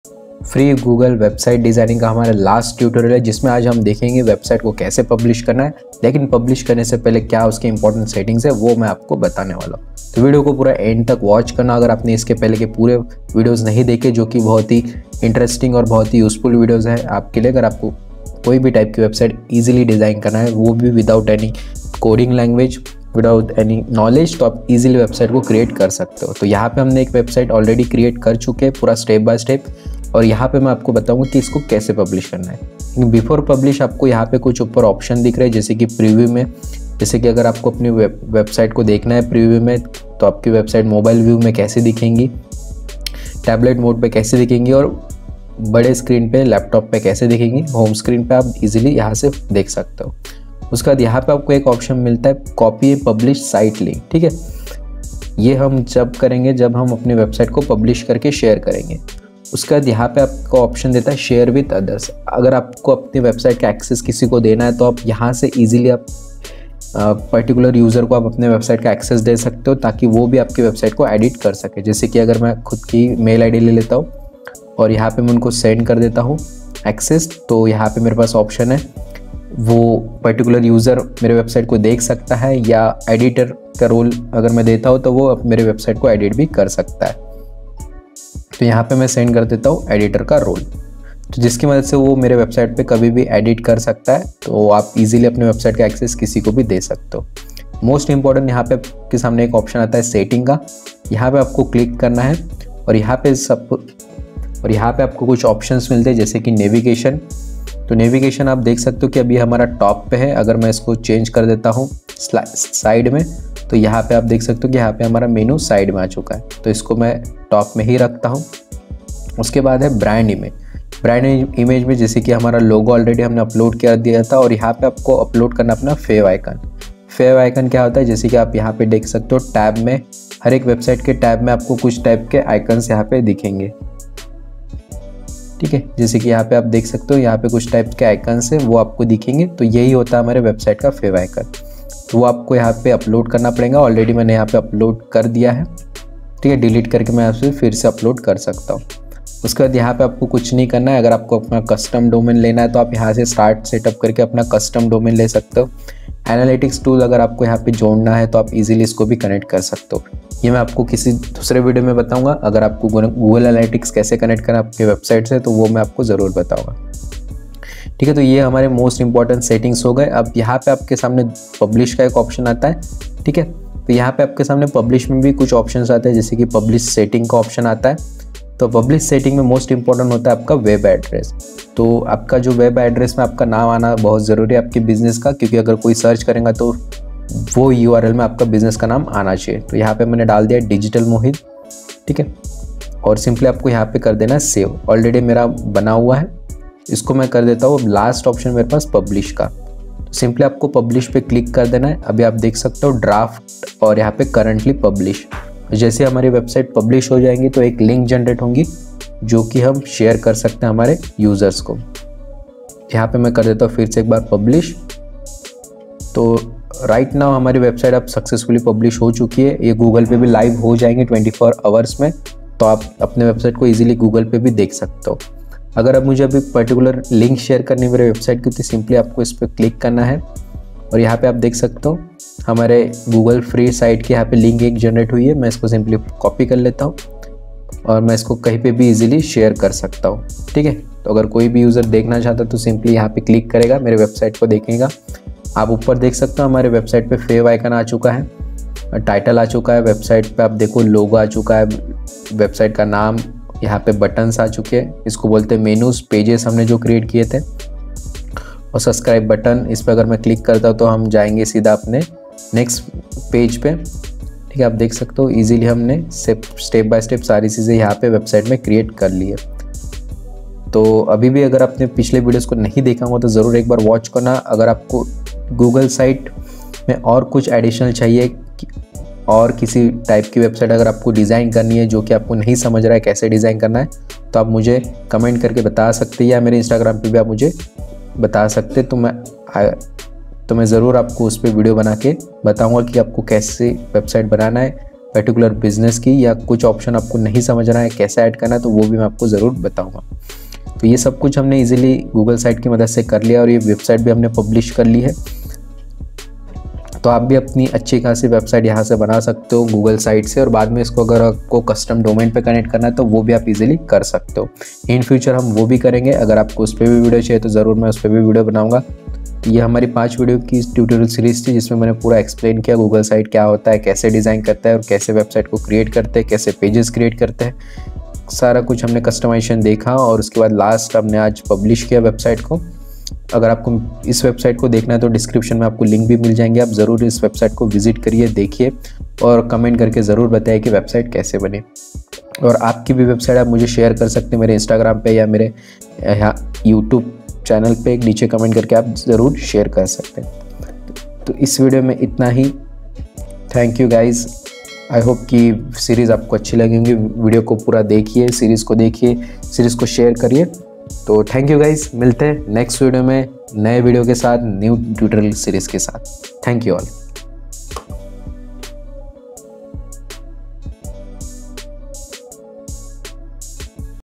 How to Create Free Website with Google Sites | Free Website Designing Tutorial in Hindi | Part-5
FREE GOOGLE SITES TUTORIAL SERIES SE AAPNE KUCH SIKHA HO TO NICHE COMMENT JARUR KARNA AUR LIKE BHI.

►  @DigitalMohit   for more awesome videos.

Equipment use-
►Boy M1 Mic